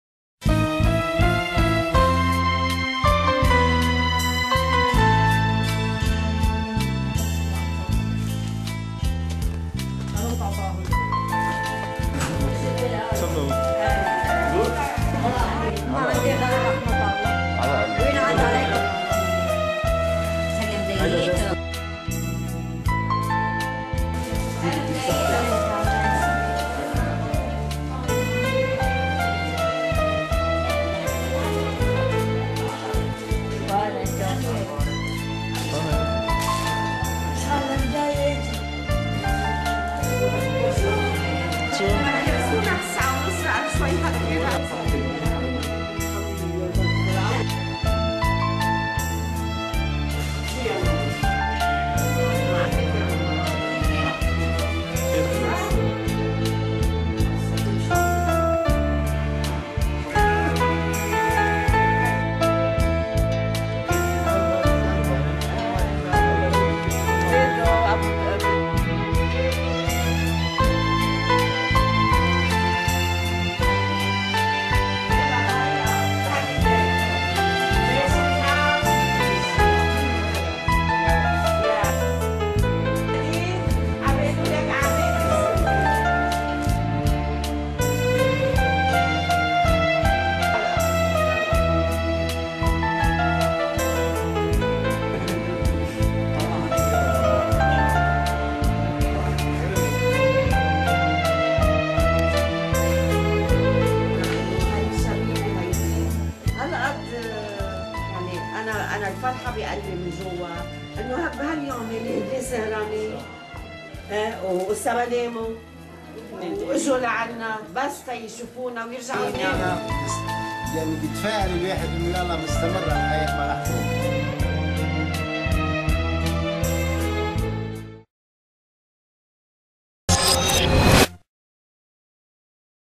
New year with hope - We Love Tripoli
New Year (2010) Morning at Beit El Shaykhoukha, Mina, Tripoli